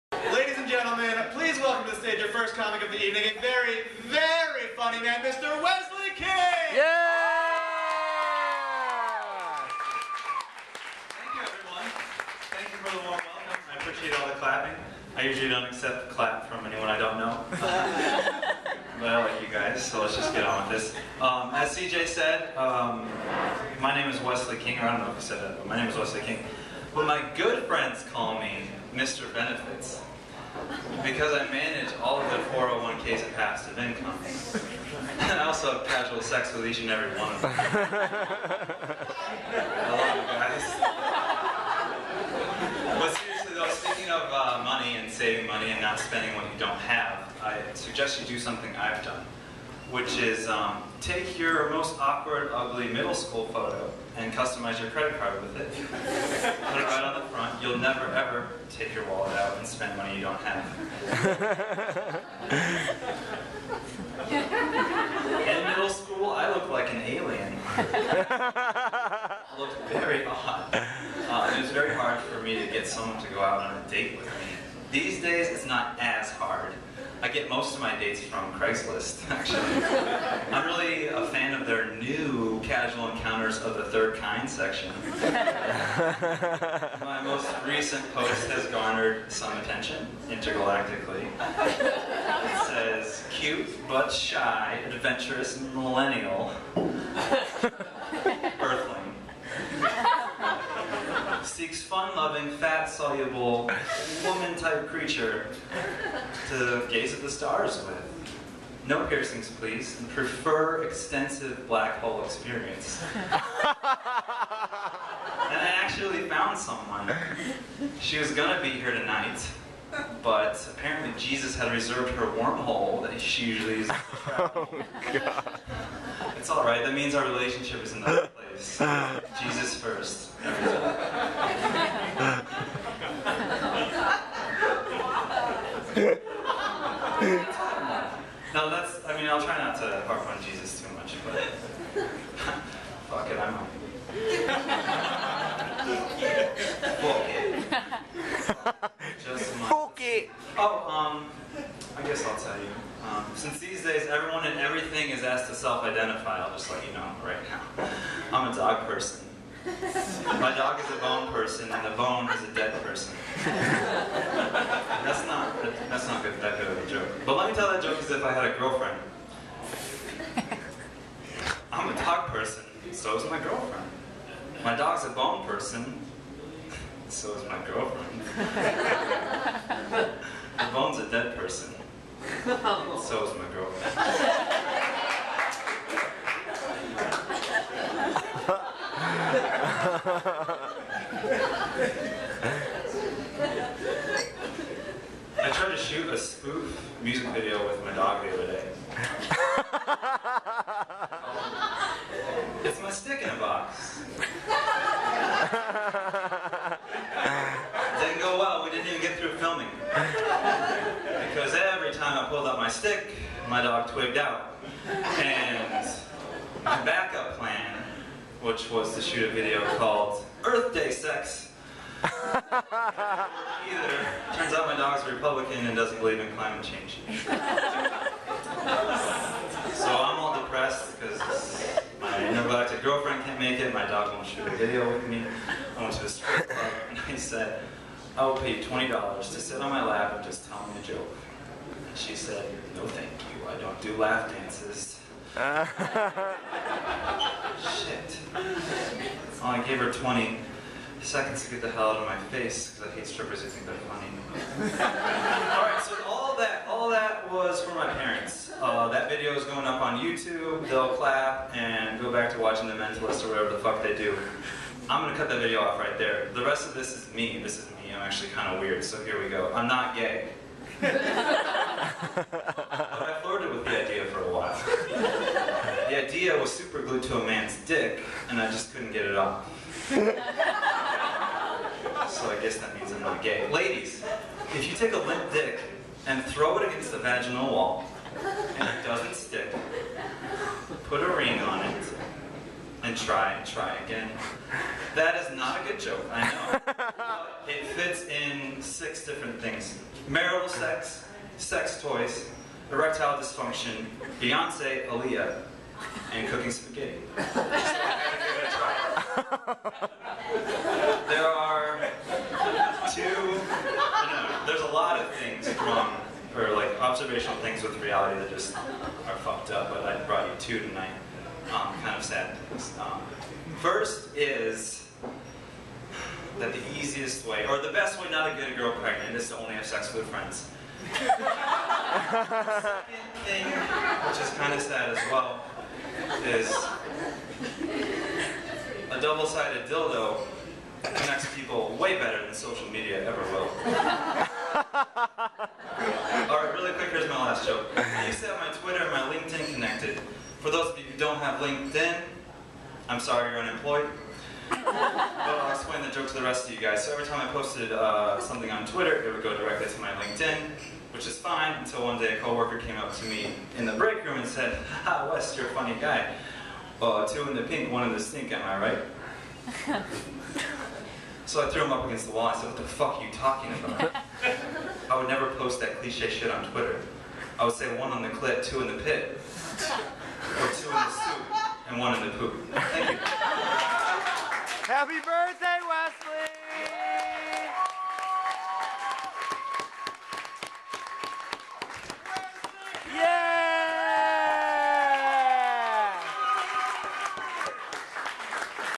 Mr. Benefits - Wesley King Standup
Friends call me Mr. Benefits.
Awkward card custom photo.
Casual encounters of the 3rd kind.
It's my "Stick in a Box" and "Earth Day Sex."
I'm a dog person.
Give me a laugh dance.
I'm not gay, but I flirted with the idea.
Sad truths about reality.
Put a ring on it and try again, try again.